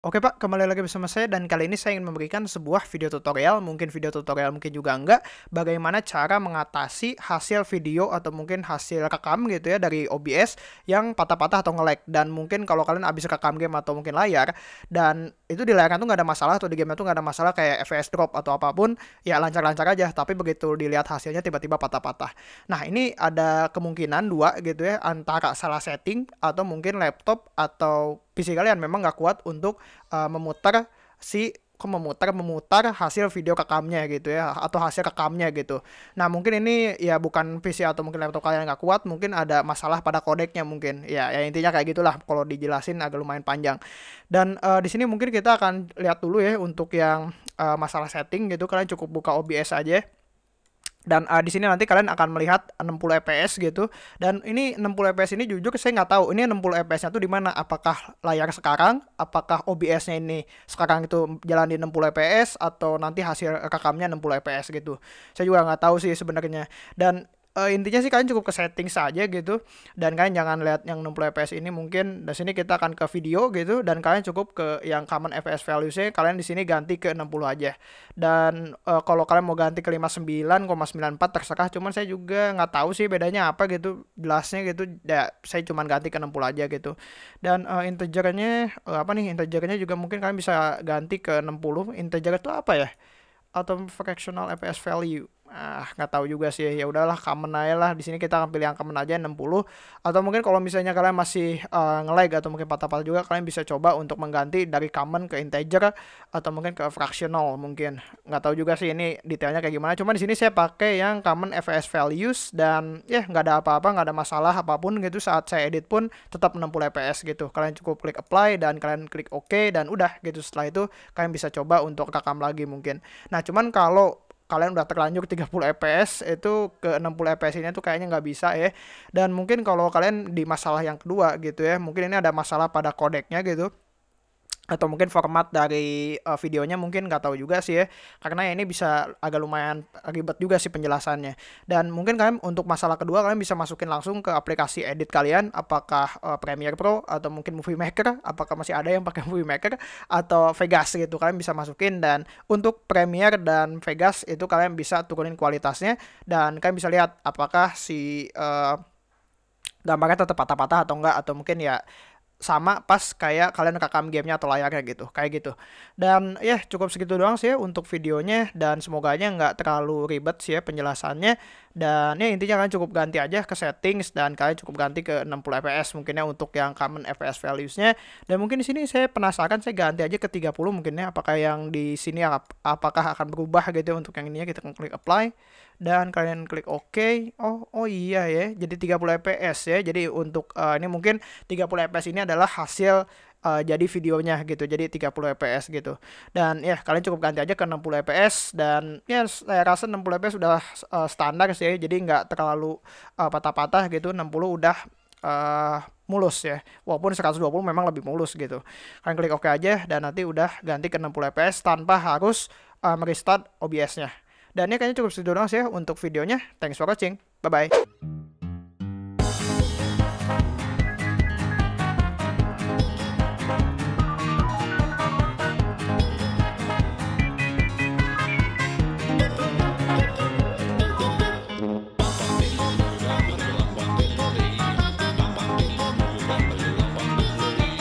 Cara Mengatasi Hasil Rekam OBS Patah-Patah!
oke pak,kali ini guwek akan memberikan sebuah video tutorial bagaimana cara mengatasi video hasil record obs yang patah-patah,dan saat kalian rekam tidak lag/patah-patah.